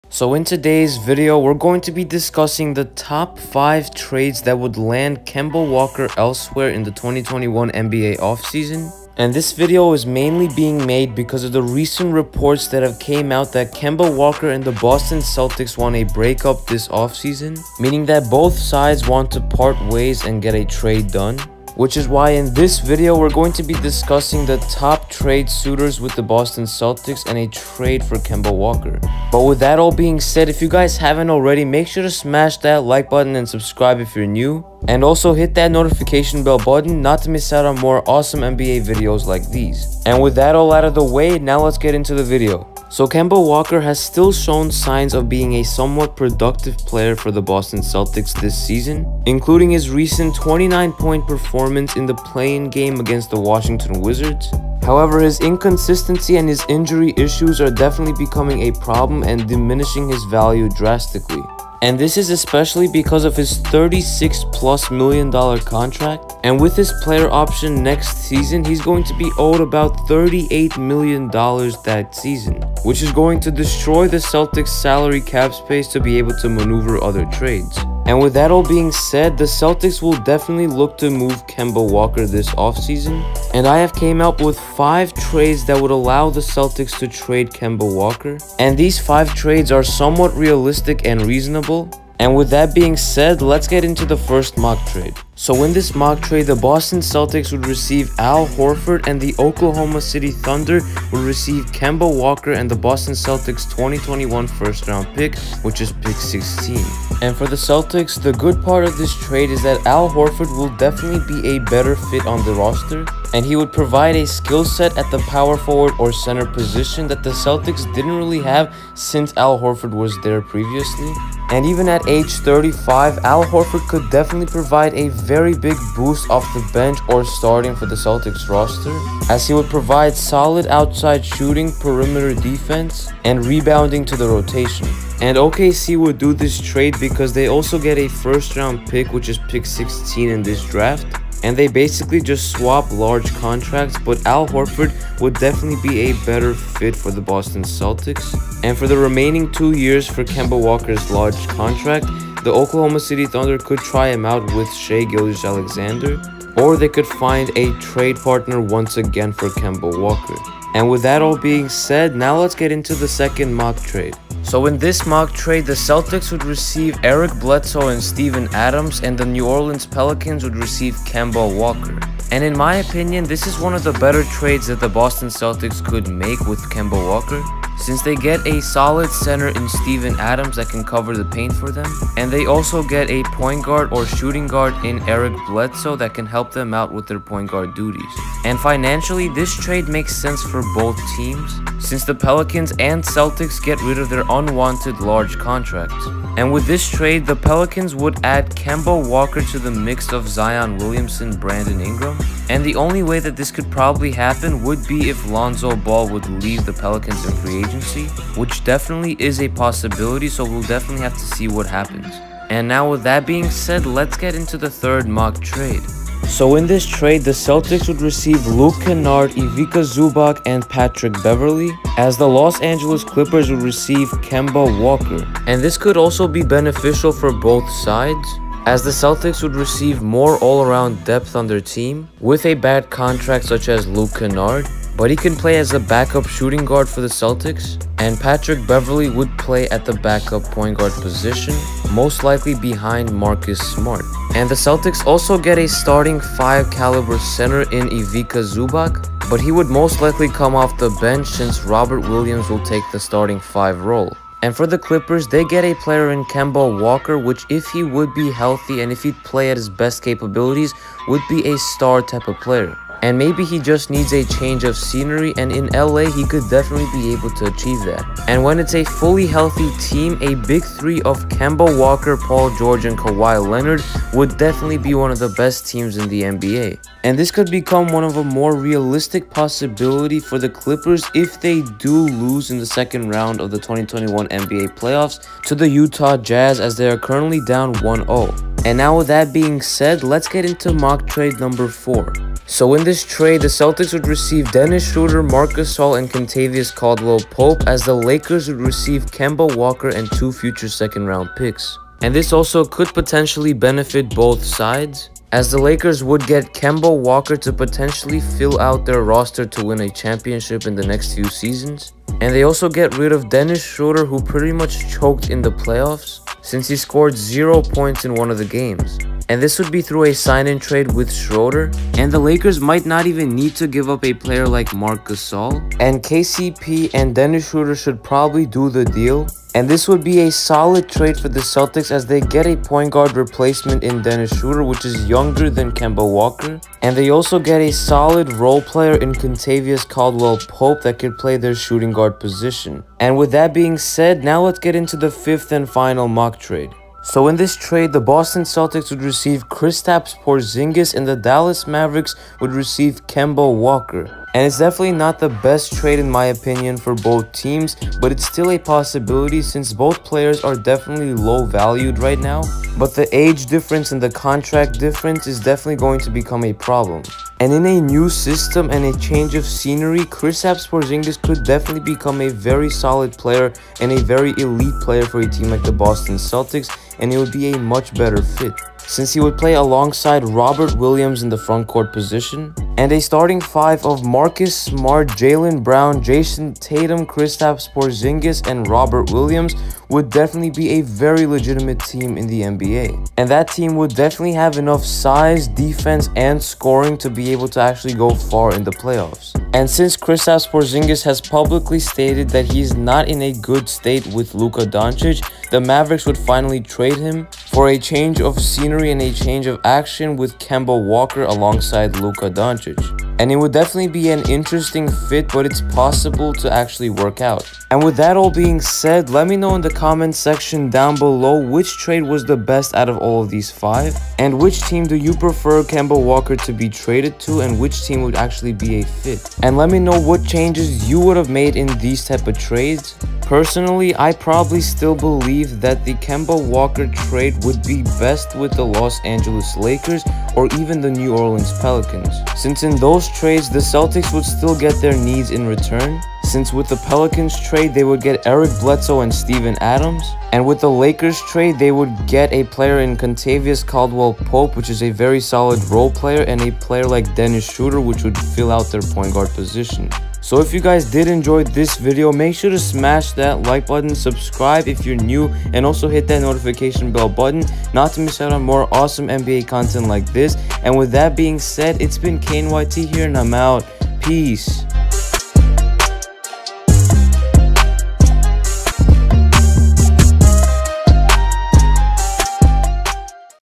5 REALISTIC Kemba Walker Trades - Leaving Boston Celtics?
5 REALISTIC Kemba Walker Trades - Leaving Boston Celtics?

With the recent reports stating that Kemba Walker and the Boston Celtics will likely end up parting ways in the 2021 NBA offseason, which team could become a trade partner with the C’s? In this video, I came up with 5 potential Kemba Walker trade scenarios that would land him on a different team. Teams like the Lakers, Clippers, OKC, Pelicans, and Mavericks could become suitors.

0:00 Intro
0:43 Situation in Boston
1:36 Mock Trade #1
2:45 Mock Trade #2
3:37 Mock Trade #3
4:53 Mock Trade #4
5:47 Mock Trade #5
6:58 Outro

Knicks TRADE RUMORS for KEMBA WALKER in 2021 NBA offseason!